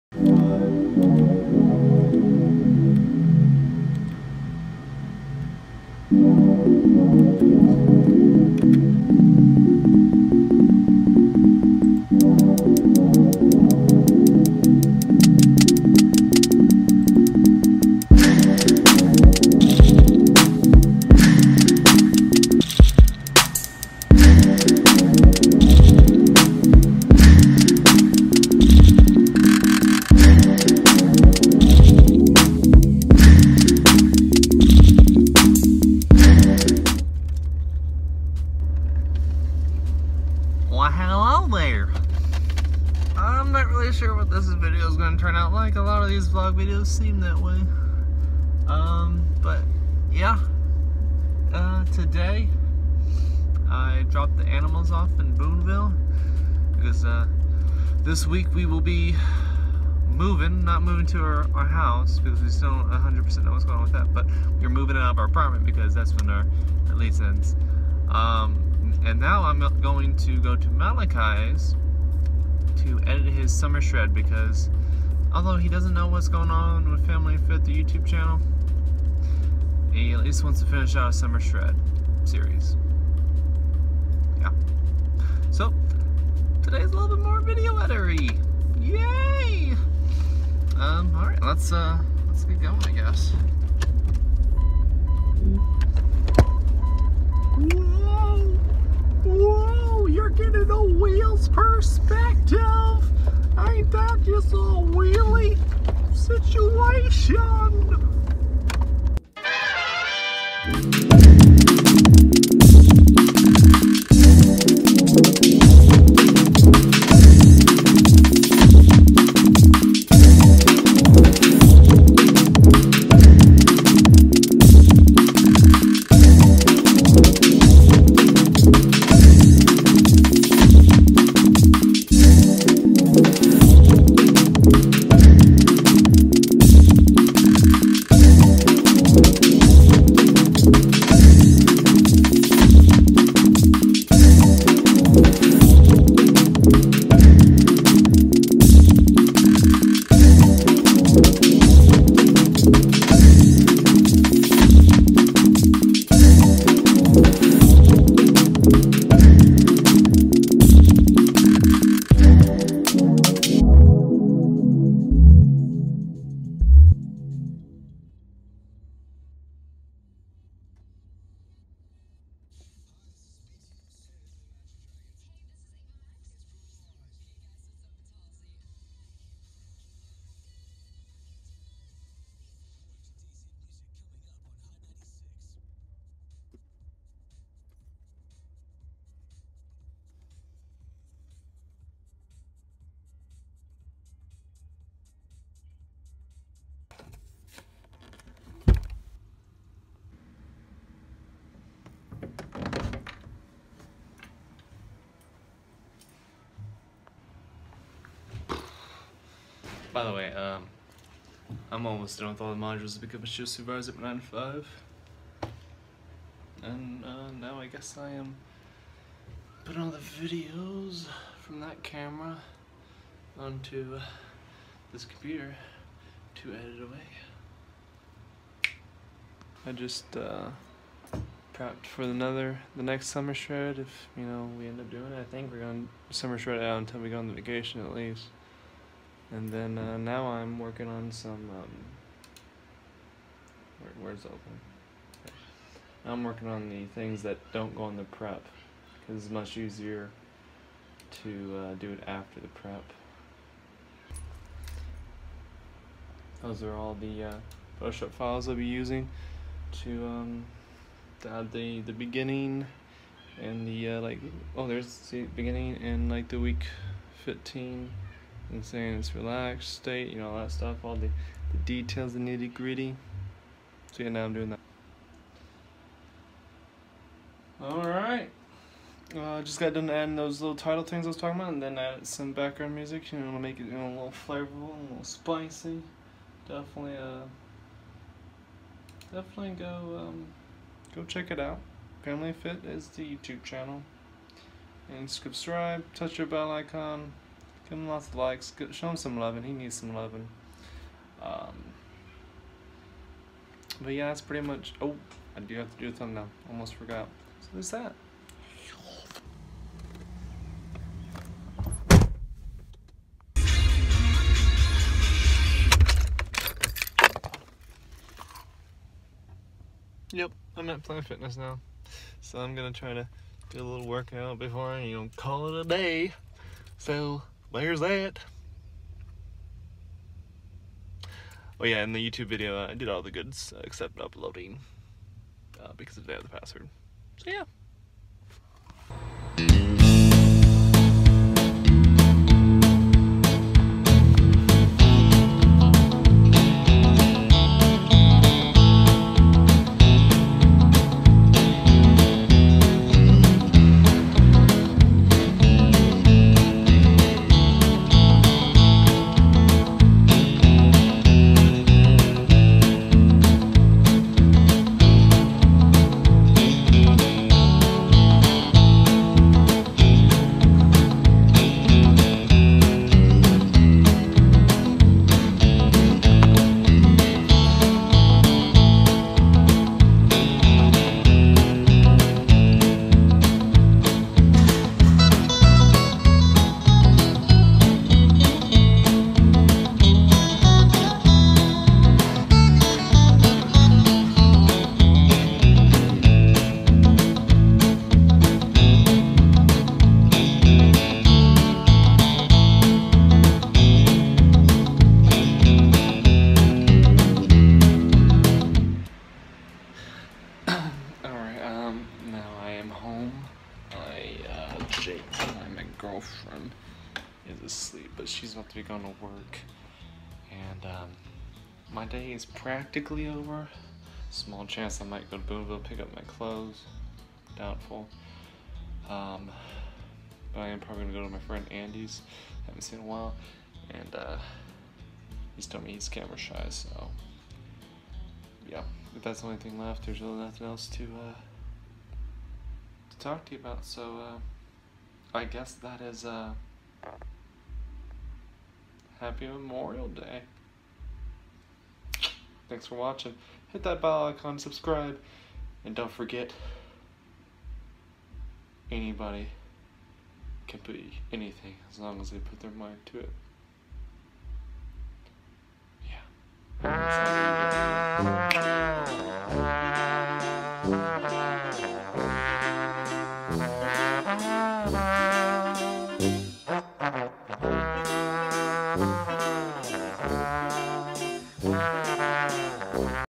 A Memorial Day as a YouTube Editor
Sorry about no end screen annotations, I forgot & can't get to a computer till an undetermined time. Vlogging it up again, this time I actually do end up editing video for YouTube. Started with the 9-5 training, ended with a trip to my friends whom is camera shy.